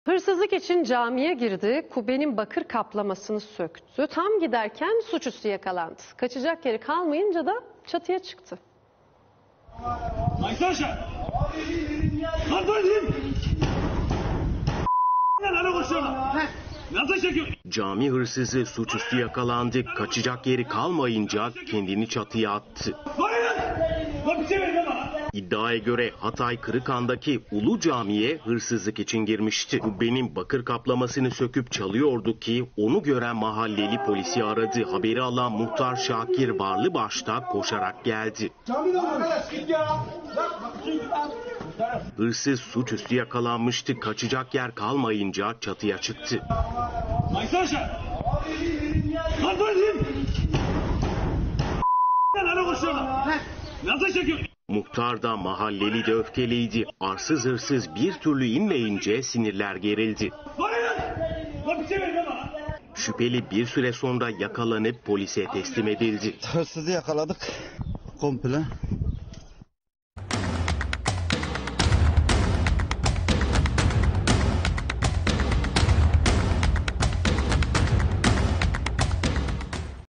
Cami hırsızına suçüstü!
HIRSIZLIK İÇİN CAMİYE GİRDİ, KUBBENİN BAKIR KAPLAMASINI SÖKTÜ. TAM GİDERKEN SUÇÜSTÜ YAKALANDI,  KAÇACAK YERİ KALMAYINCA DA ÇATIYA ÇIKTI!